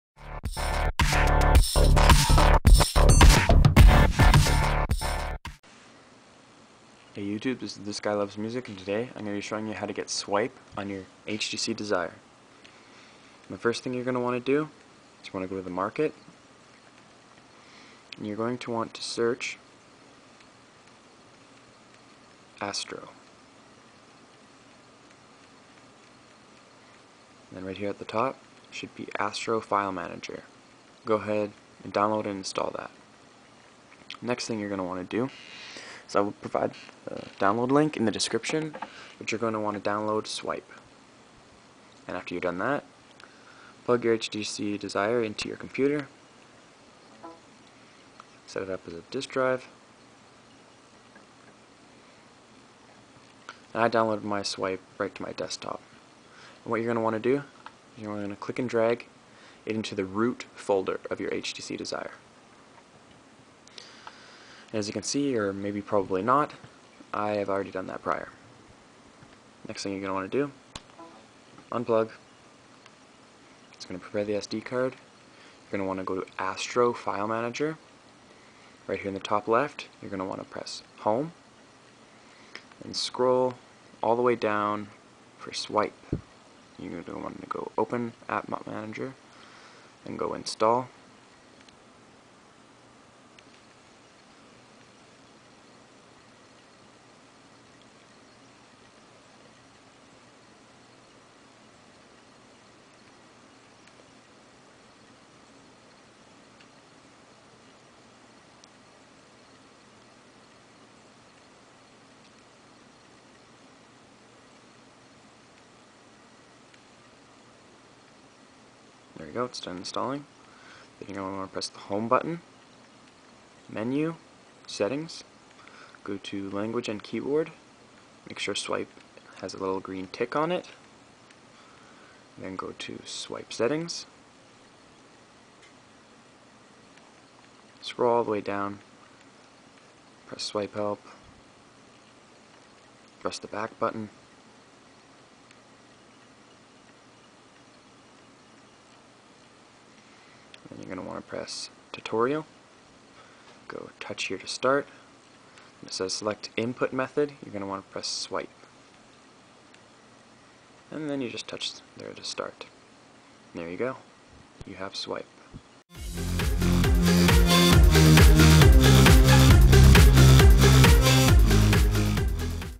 HOW TO- Put Swype on your HTC Desire
Telus HTC Desire running Android 2.2 (FroYo) with Samsung Swype

UPDATE: Jan 15 (1,000 views) Feb 6 (2,000 views) Feb 25 (3,000 views) Mar 19 (4,000 views) April 11 (5,000 views) May 6 (6,000 views)

My camera is a piece...I know that, the only decent camera I have is on my Desire, and I couldn't use that, for obvious reasons